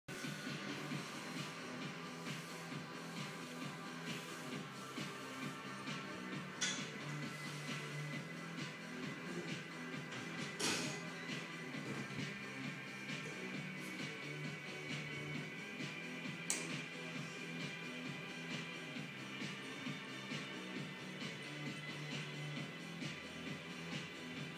Dumbbell sword draw
Rotator cuff exercise building rotational strength into shoulder stabilizers